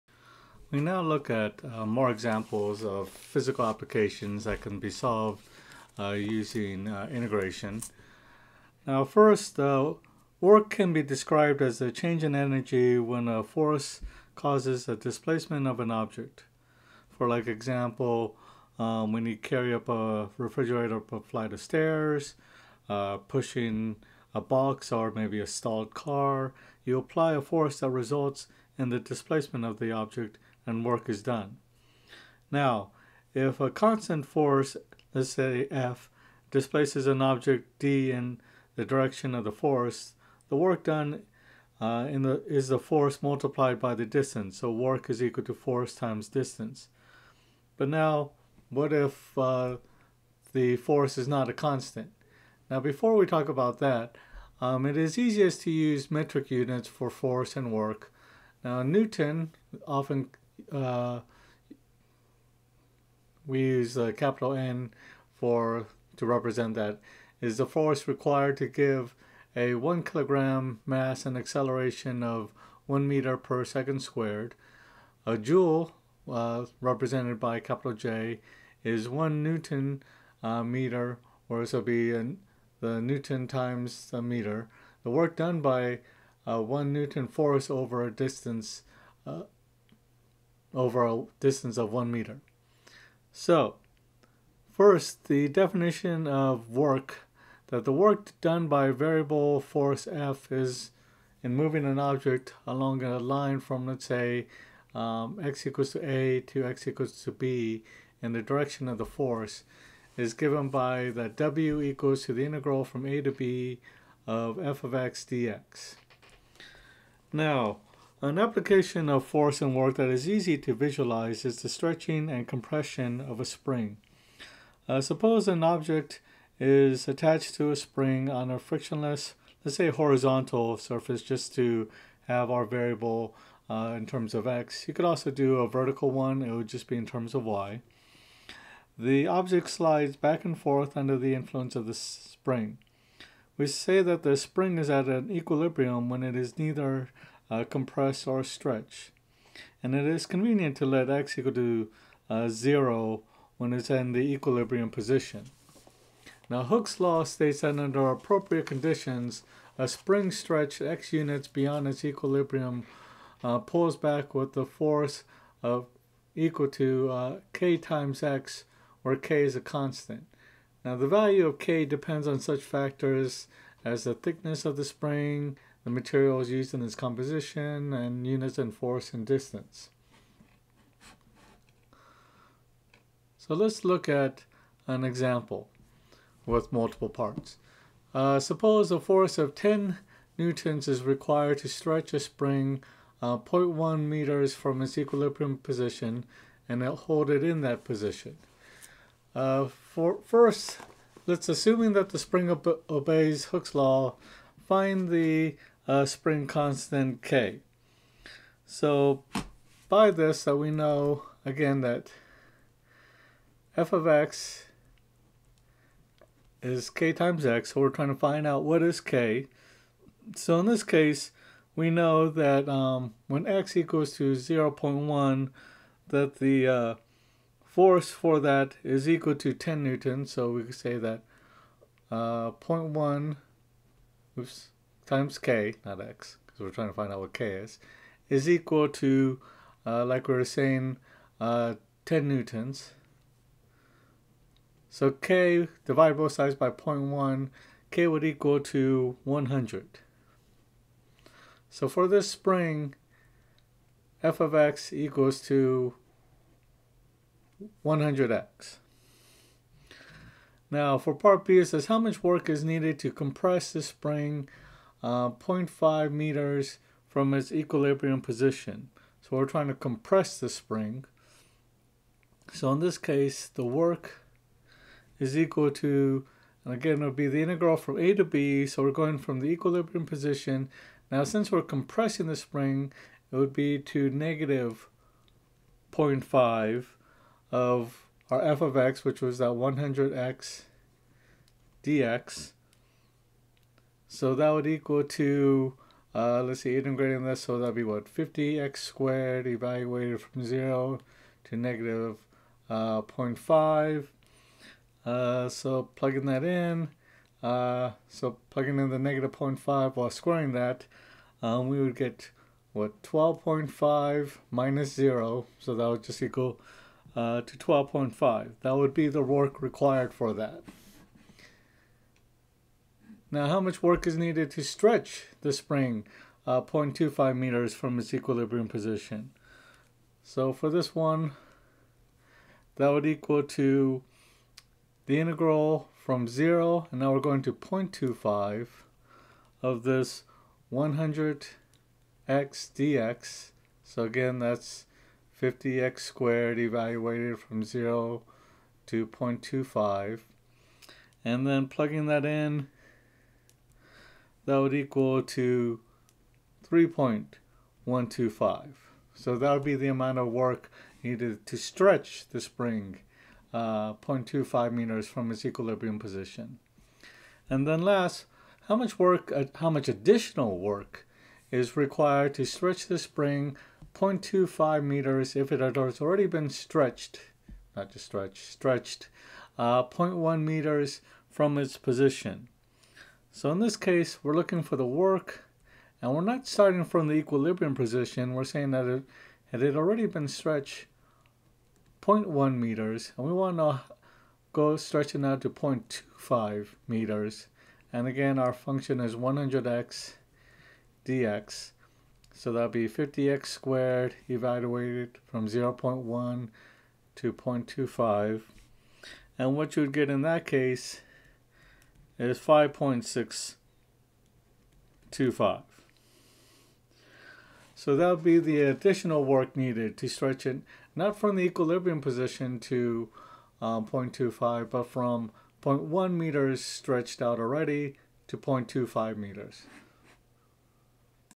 Work and Stretching a Spring
An introduction to work and the example of stretching/compressing a spring